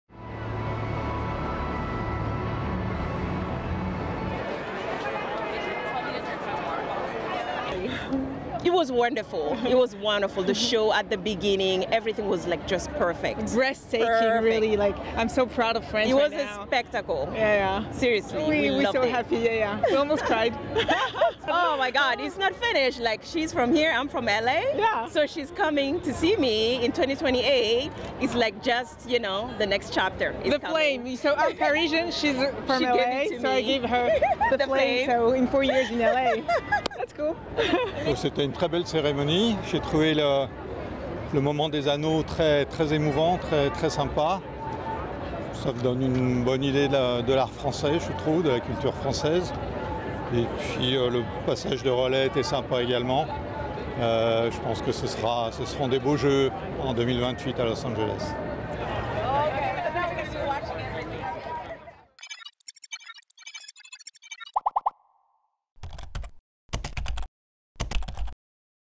Com encerramento dos Jogos de Paris, fãs das Olimpíadas estão ansiosos pelo ‘próximo capítulo’
Ao saírem do estádio no início da madrugada de segunda-feira (12 de agosto), espectadores da cerimônia de encerramento das Olimpíadas disseram que já estão de olho nos próximos Jogos Olímpicos, após Tom Cruise entregar a bandeira olímpica para Los Angeles, marcando o fim de Paris 2024.
IMAGENS REUTERS